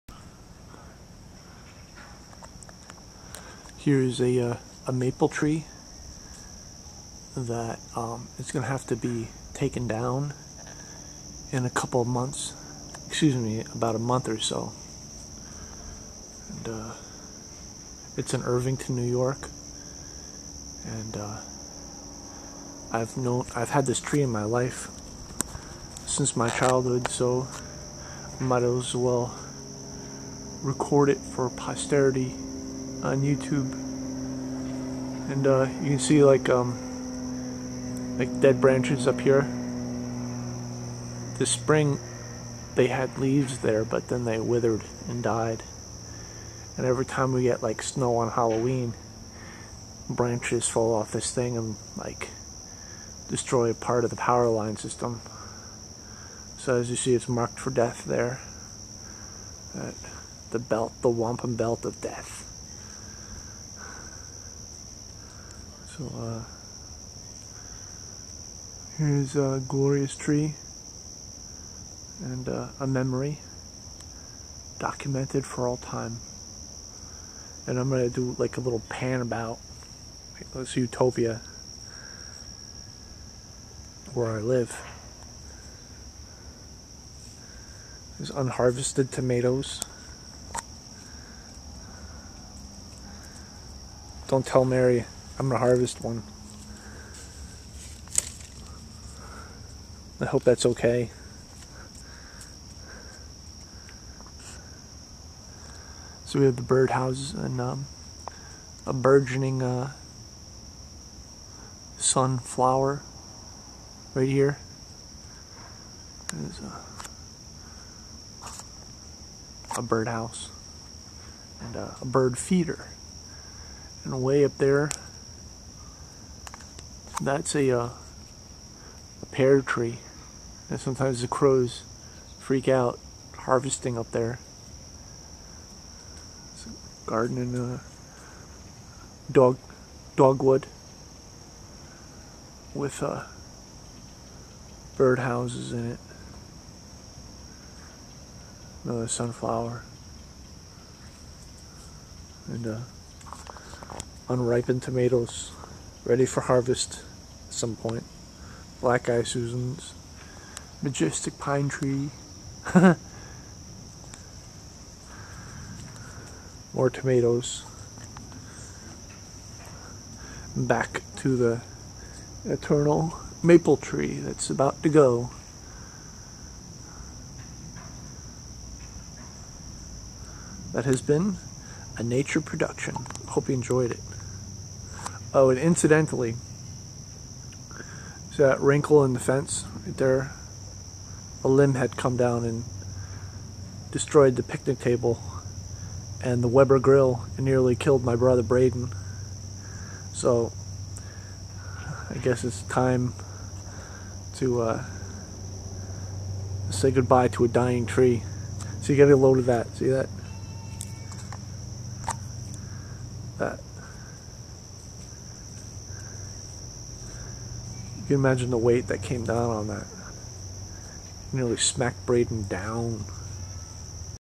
A Maple Tree Of Irvington New York
A maple tree in Irvington. A tree that will have to be taken down in September 2013. It turned out to be a very sick large tree that had to be removed.